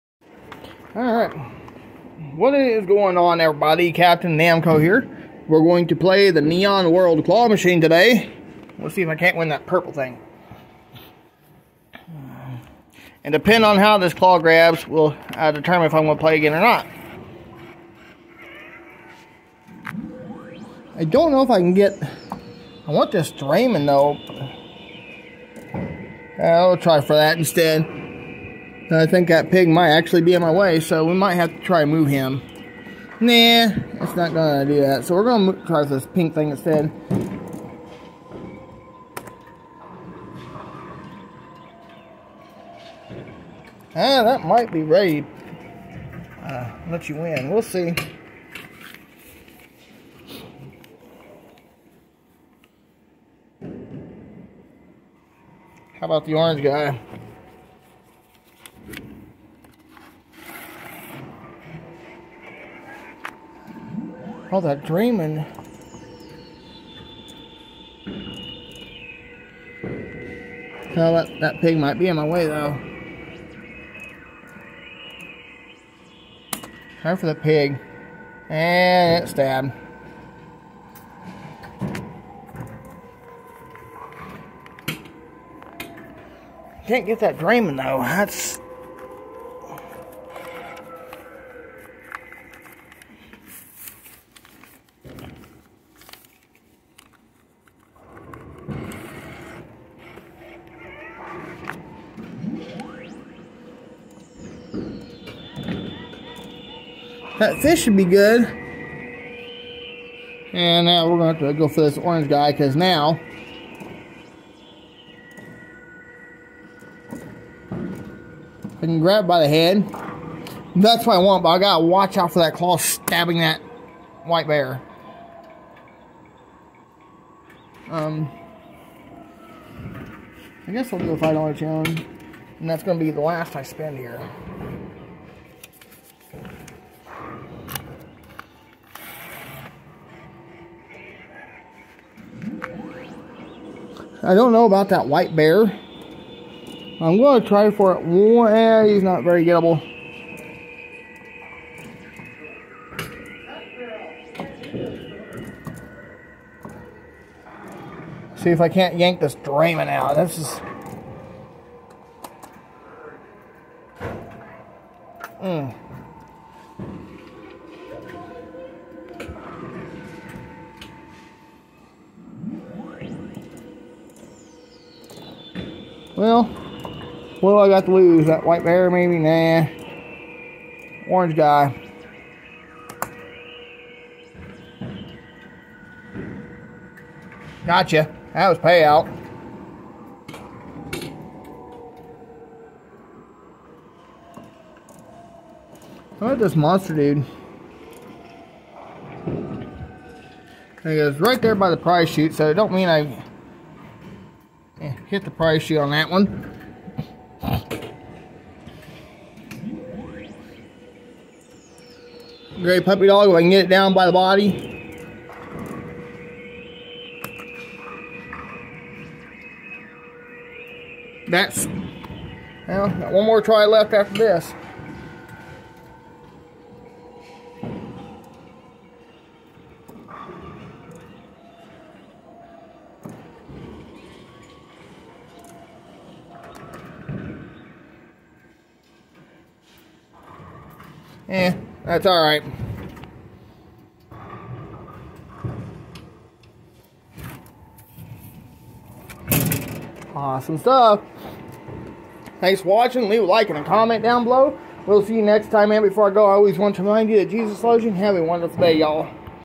Neon World Claw Machine @Silver Dollar Lanes
good afternoon everyone Captain Namco here this was filmed on Monday April 3rd at my local bowling alley so it's been a few weeks I hope you enjoy thanks for watching and before I go I always want to remind you that Jesus loves you